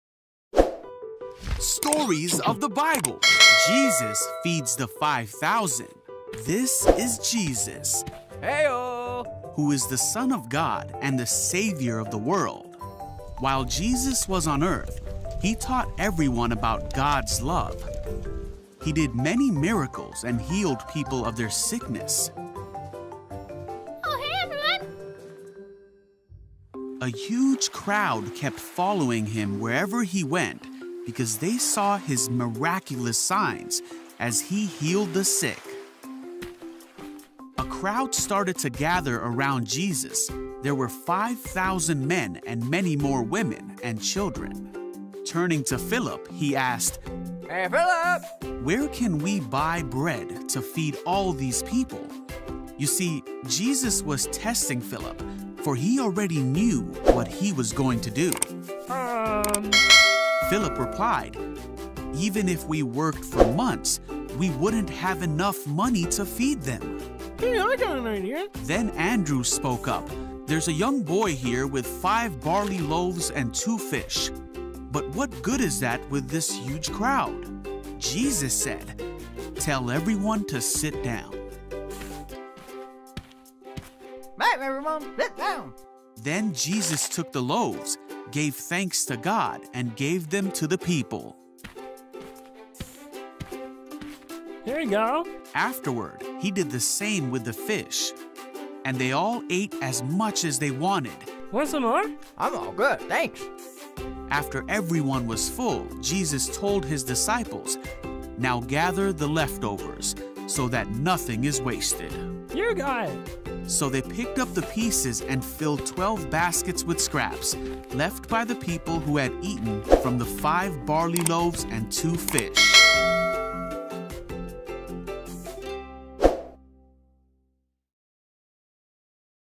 Jesus Feeds the 5,000 (Matthew 14 13-21) - Bible Stories
Bible Stories
Cartoons Bible Stories
YakuKids Tv